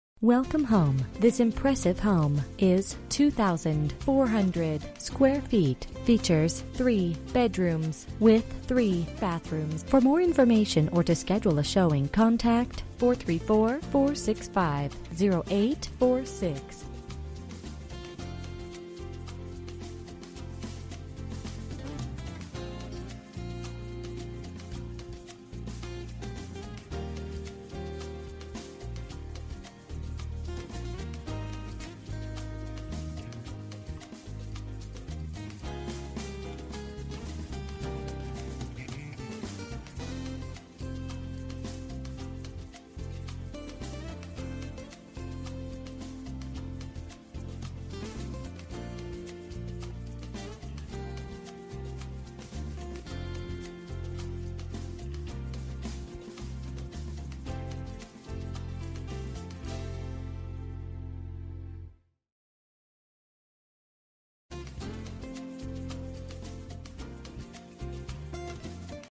Residential for sale - 7 Mills Hollow Ln, Ruckersville, VA 22968
OPEN HOUSE SUNDAY 5/26/19 1-3 PM. New Home, Ready in 30 Days, New Road installed, (LOT 1 is on the left), only 5 Minutes to Food Lion, Lowes & Walmart & close to NGIC. Watch the walk through video of this model with one level living, all bedrooms and washer/dryer hook-ups on the main level! Wood Floors on the main level, vaulted ceiling in great room. Hardie-Plank siding, Full basement with two Car Garage, Family Room, 3rd Full Bath and large Laundry Room. Gorgeous 2 acre pastoral lot with views in the country.

Bedrooms: 3
Bathrooms: 3
Square Feet: 2400
Price: $334,950

06/03/2019 01:44:12 am

Last modified: